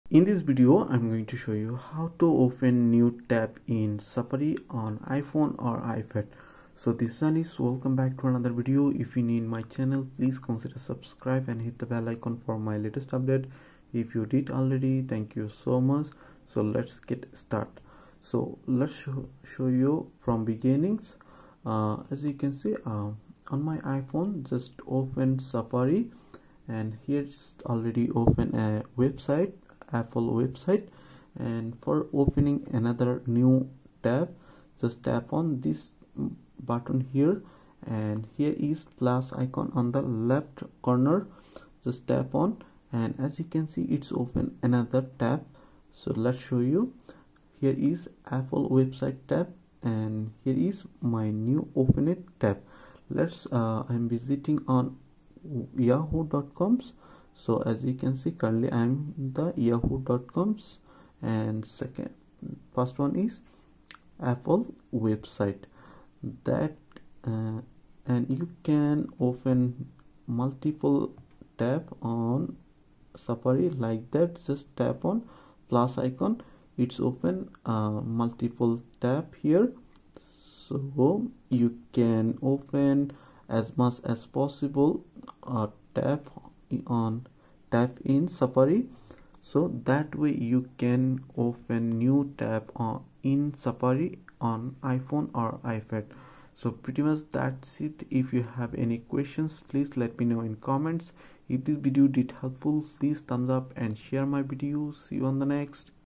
How to Open New Tab in Safari on iPhone | Full Guide
In this video i show you how to open new tab in Safari on iPhone